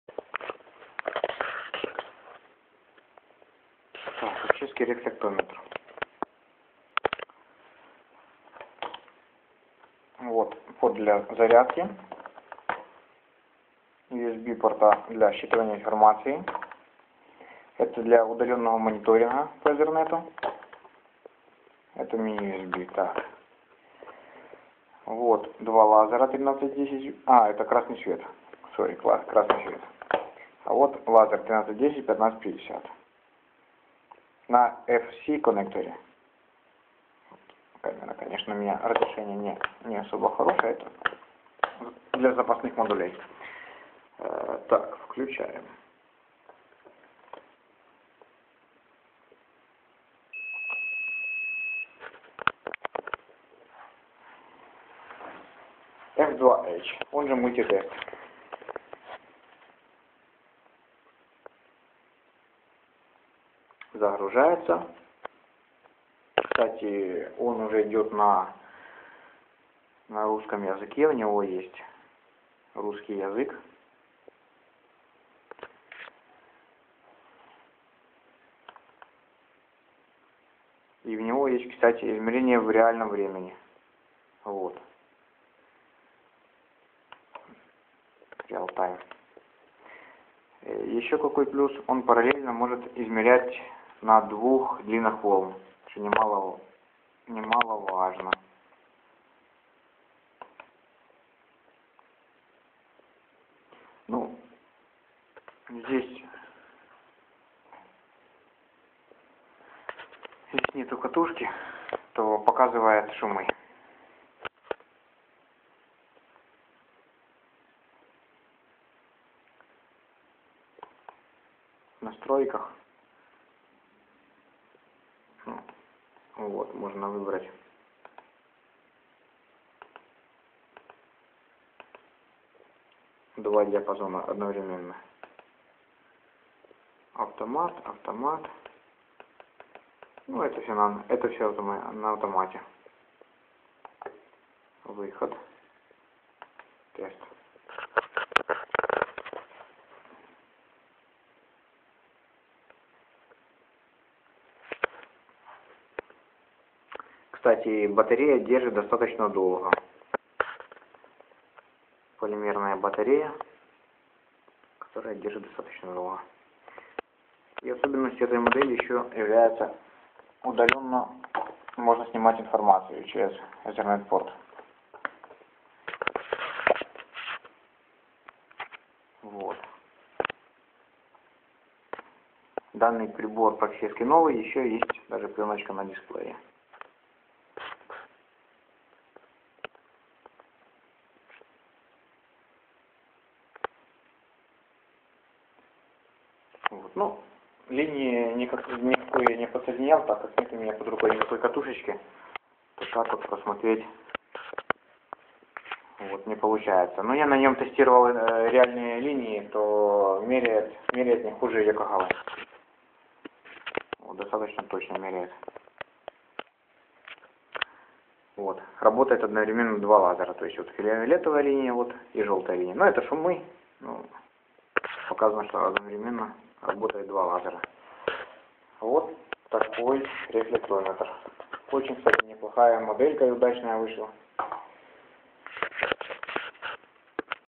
F2H FHO 5000 Grandway (Китай) Оптический рефлектометр
F2H FHO 5000 Grandway (Китай) Оптический рефлектометр. Одновременное измерение параметров оптической трассы на двух длинах волн 1310нм и 1550нм